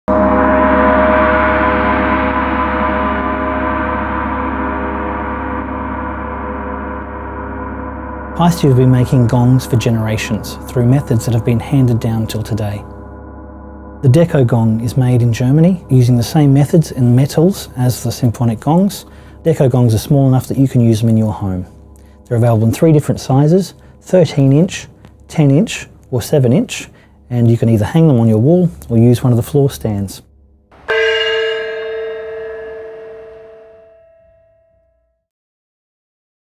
Paiste Deco Gong
The Paiste Deco Gongs are a range of small scale gongs. Manufactured in Germany using the same metals and techniques as the large Symphonic gongs they are true musical gongs with complex harmonics. The Deco Gongs make a unique and decorative addition to your personal or musical environment. Available in 7", 10" and 13" varities with either a floor stand or wall hanger.

Special thanks to Bakehouse Studios for the Recording Location

The audio in this video was recorded with a Sehnhiser Wireless Lapel, Rode NT5 Pencil Condenser into a Steinberg UR824 Interface and Cubase Pro 8.